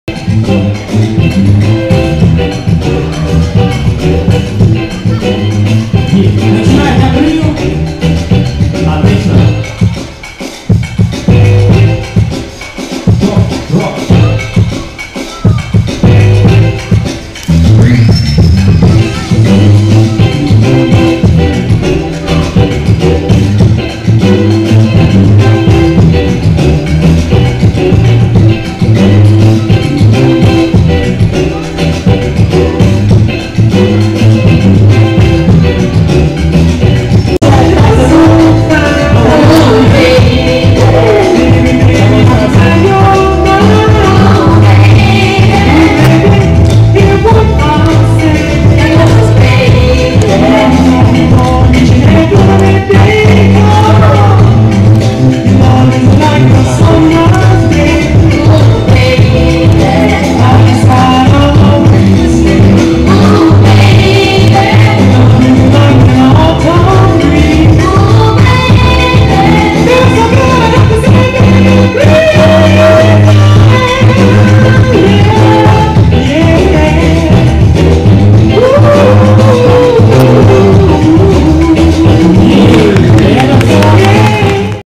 Tasya Rock vs. Акрилл - "Make It Funky 5"
Juniors 1 vs. 1 "Full Style System"  (до 4-х лет танцевального опыта)
1/8 Final - Top Rock
Win: Tasya Rock

Дом молодёжи "Атлант"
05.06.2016